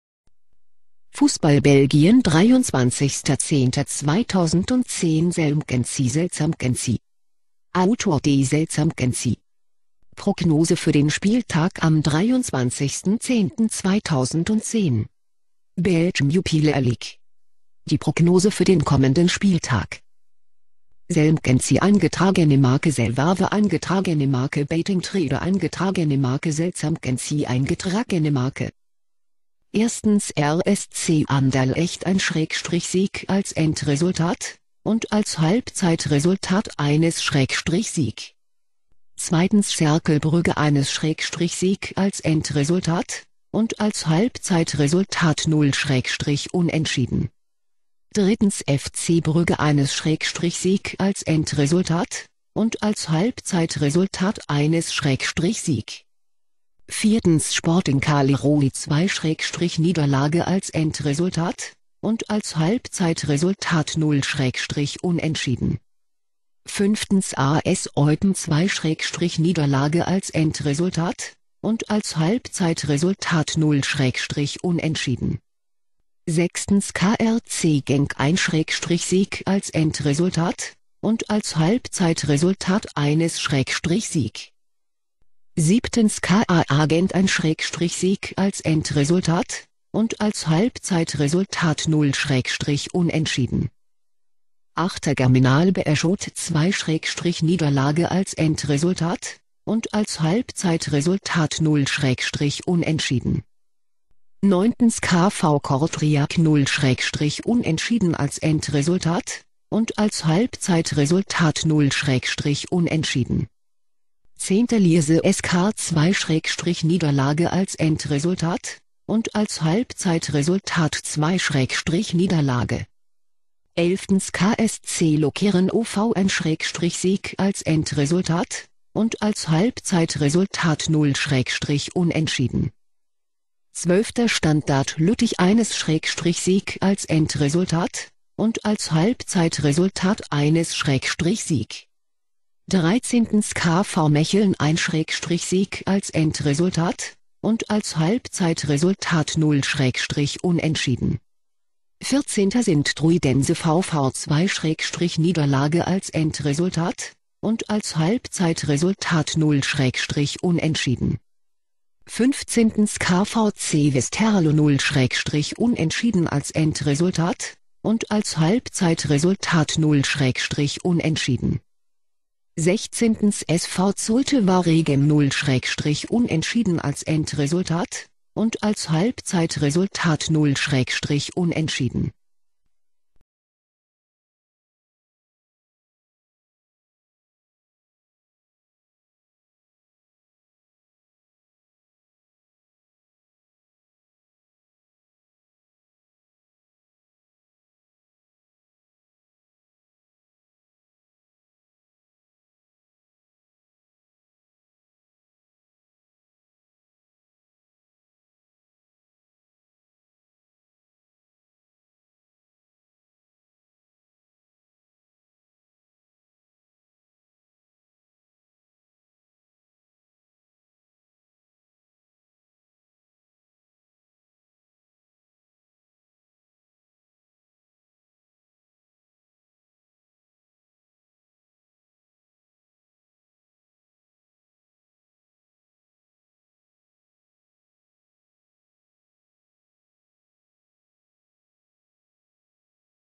Fussball Belgien 23.10.2010 SelMcKenzie Selzer-McKenzie
Author D.Selzer-McKenzie
Prognose für den Spieltag am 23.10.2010
Belgium Jupiler League
Die Prognose für den kommenden Spieltag:
SelMcKenzie®    SelWave®    BettingTrader®    Selzer-McKenzie®

1.  RSC Anderlecht  1 / Sieg  als End-Resultat, und als Halbzeit-Resultat  1 / Sieg
2.  Cercle Brügge  1 / Sieg  als End-Resultat, und als Halbzeit-Resultat  0 / Unentschieden
3.  FC Brügge  1 / Sieg  als End-Resultat, und als Halbzeit-Resultat  1 / Sieg
4.  Sporting Charleroi  2 / Niederlage  als End-Resultat, und als Halbzeit-Resultat  0 / Unentschieden
5.  AS Eupen  2 / Niederlage  als End-Resultat, und als Halbzeit-Resultat  0 / Unentschieden
6.  KRC Genk  1 / Sieg  als End-Resultat, und als Halbzeit-Resultat  1 / Sieg
7.  KAA Gent  1 / Sieg  als End-Resultat, und als Halbzeit-Resultat  0 / Unentschieden
8.  Germinal Beerschot  2 / Niederlage  als End-Resultat, und als Halbzeit-Resultat  0 / Unentschieden
9.  KV Kortrijk  0 / Unentschieden  als End-Resultat, und als Halbzeit-Resultat  0 / Unentschieden
10.  Lierse SK  2 / Niederlage  als End-Resultat, und als Halbzeit-Resultat  2 / Niederlage
11.  KSC Lokeren OV  1 / Sieg  als End-Resultat, und als Halbzeit-Resultat  0 / Unentschieden
12.  Standard Lüttich  1 / Sieg  als End-Resultat, und als Halbzeit-Resultat  1 / Sieg
13.  KV Mechelen  1 / Sieg  als End-Resultat, und als Halbzeit-Resultat  0 / Unentschieden
14.  Sint-Truidense VV  2 / Niederlage  als End-Resultat, und als Halbzeit-Resultat  0 / Unentschieden
15.  KVC Westerlo  0 / Unentschieden  als End-Resultat, und als Halbzeit-Resultat  0 / Unentschieden
16.  SV Zulte-Waregem  0 / Unentschieden  als End-Resultat, und als Halbzeit-Resultat  0 / Unentschieden